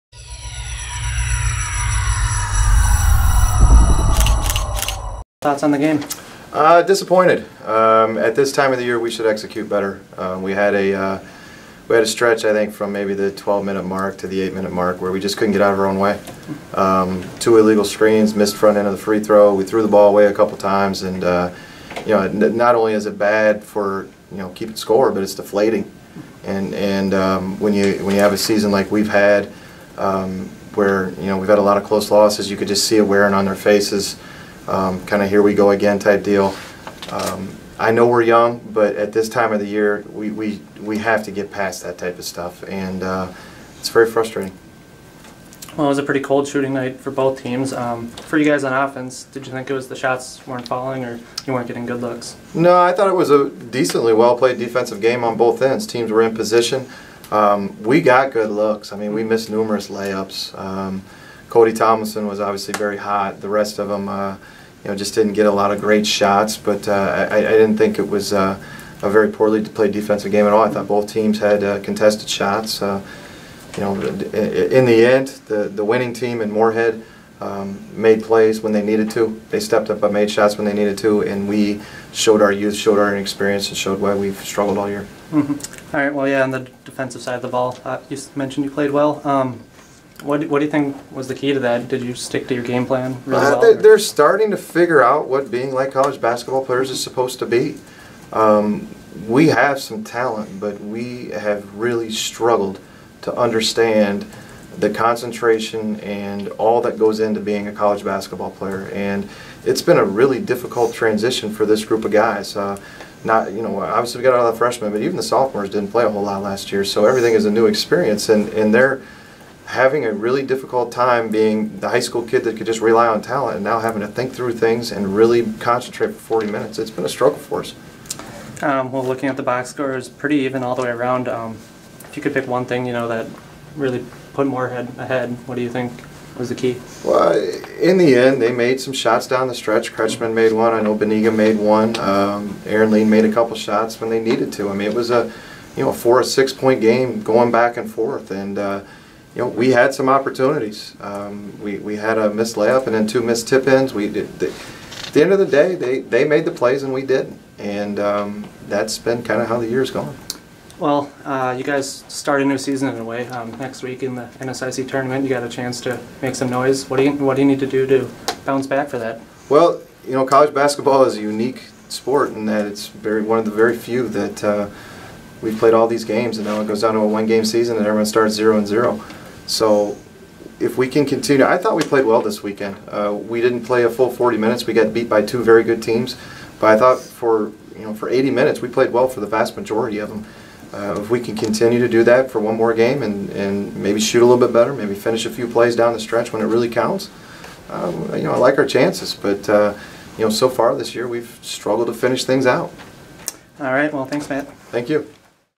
MSU Moorhead 62, UMD 56 Post Game Interview
UMD men's basketball head coach Matt Bowen discusses the 62-56 loss to MSU Moorhead.